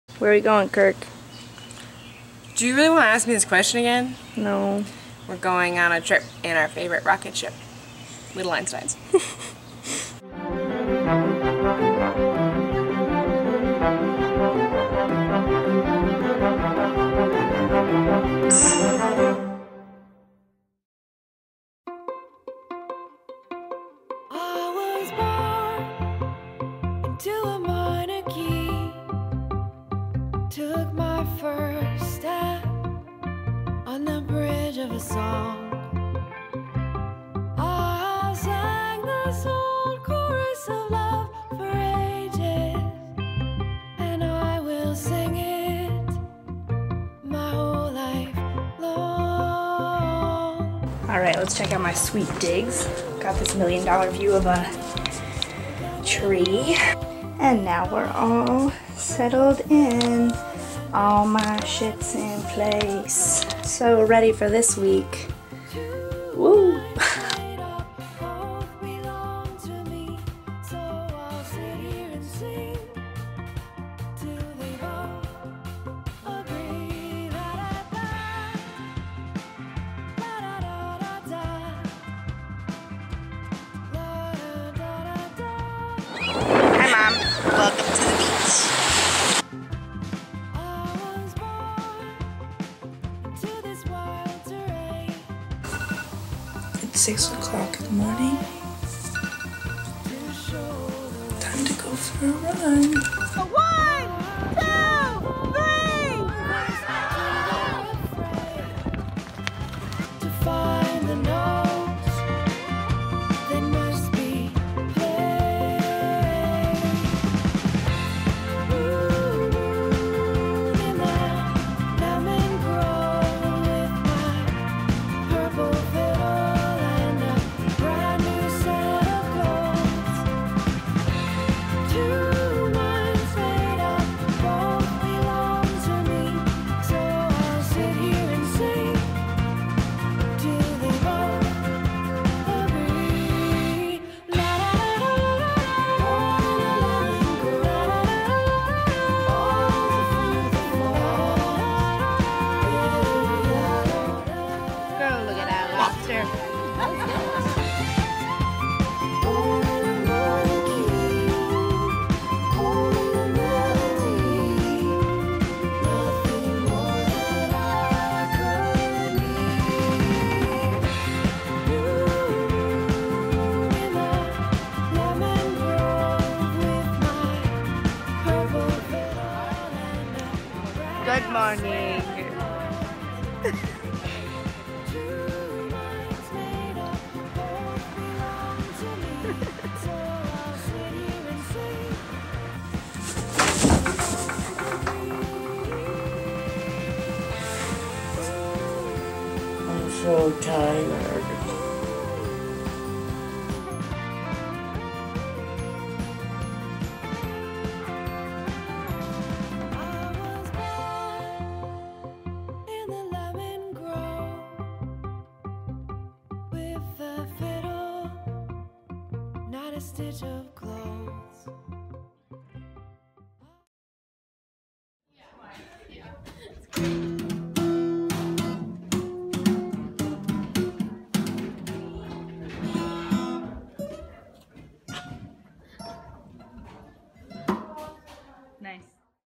Welcome to VacationLand!
I spent a week as a counselor for middle schoolers at Ferry Beach, here is a little taste of what we did!!